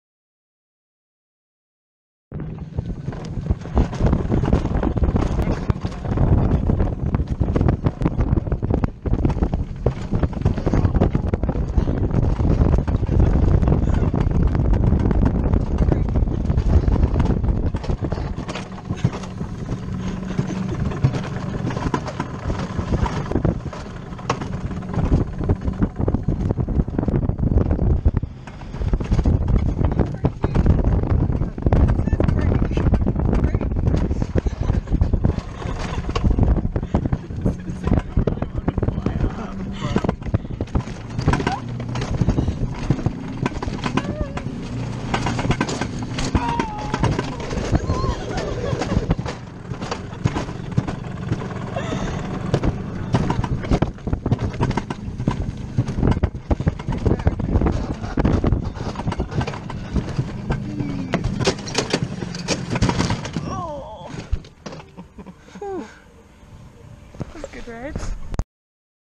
ALLGÄU COASTER SÖLLERECK
Alpine Coaster located at the base of the Söllereck. €5/Person.  Discounted with AllgäuWalserCard.